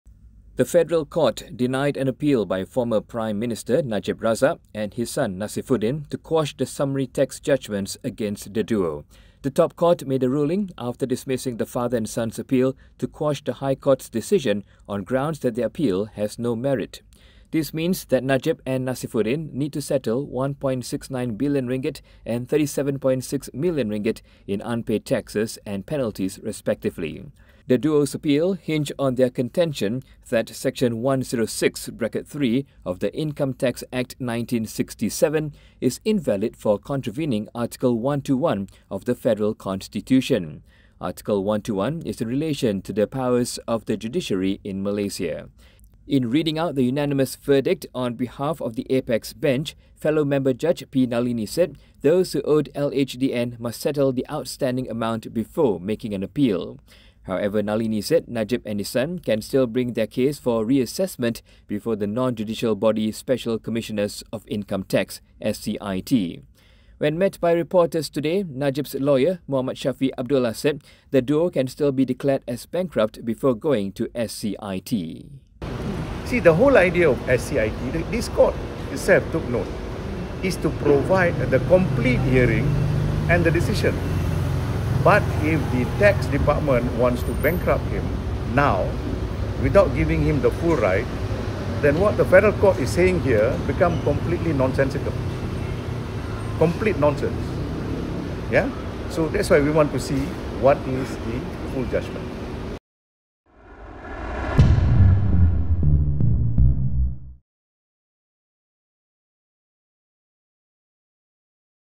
Apex court rules Najib and son to pay RM1.69b in unpaid taxes
The Federal Court denied an appeal by former prime minister Najib Abdul Razak and his son to quash the summary tax judgments against the duo for RM1.69 billion and RM36.7 million respectively.